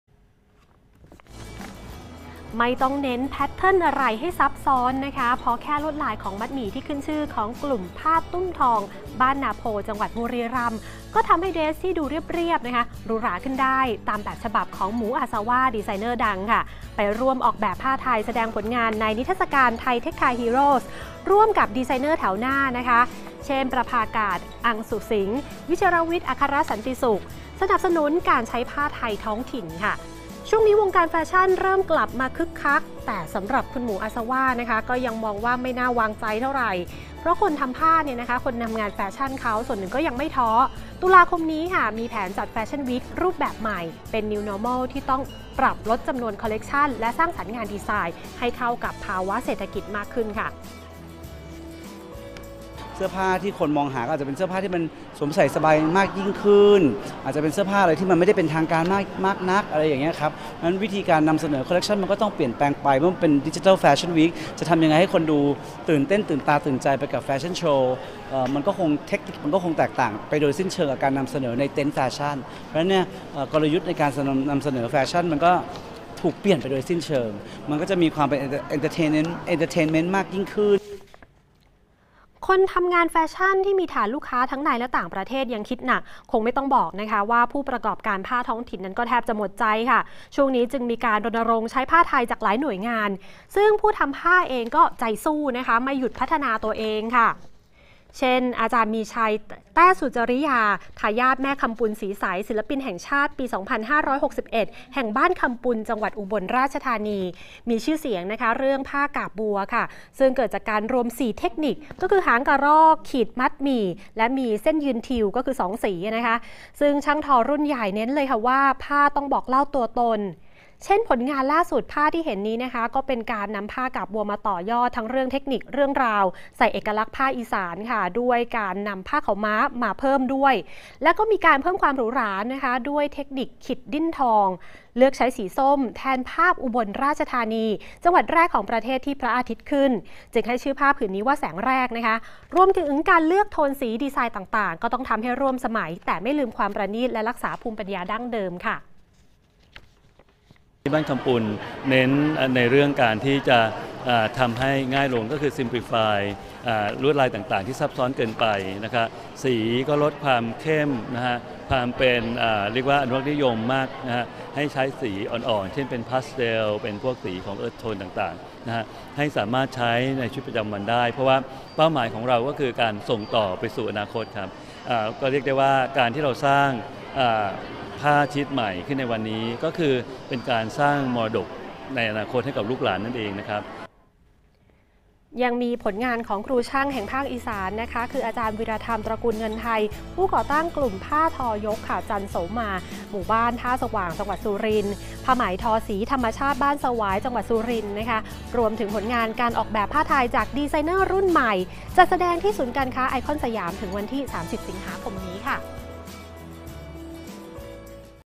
โอกาสคนวงการแฟชั่นหลังโควิด-19 คลี่คลาย
ในช่วงโควิด-19 ที่วงการแฟชั่นและงานผ้าไทยซบเซา คนในวงการก็ร่วมกันรันแคมเปญ saveTHAIwearTHAI กันอย่างต่อเนื่อง จนมาถึงช่วงนี้คงต้องเรียกว่าเป็นโอกาสที่ต้องคว้า เพราะหลายหน่วยงานเริ่มกลับมาส่งเสริมผ้าไทยกันอย่างคึกคึก รวมถึงงานนี้ Thai textile heroes ที่ได้ดีไซเนอร์ดังจับมือสร้างสรรค์ร่วมกับครูช่างท้องถิ่น